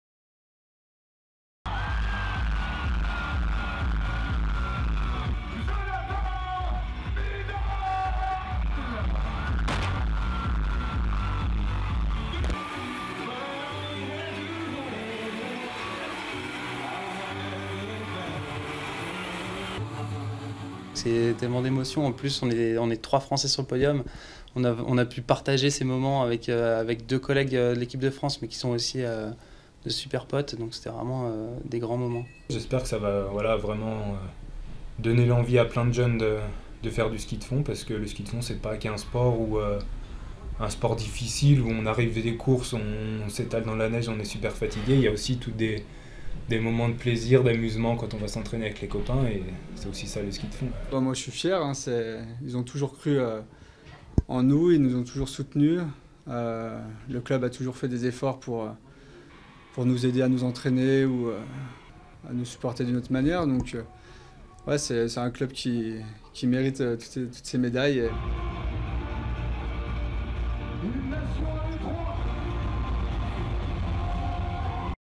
JO-2014: des médaillés français de retour en Haute-Savoie
La station du Grand-Bornand en Haute Savoie célébrait mardi 25 février le retour de ses trois médaillés aux Jeux Olympiques de Sotchi: Jonathan Midol, Steve Missillier et Ivan Perrillat. Plusieurs milliers de personnes s'étaient réunies dans le village pour acclamer les champions. Durée: 01:15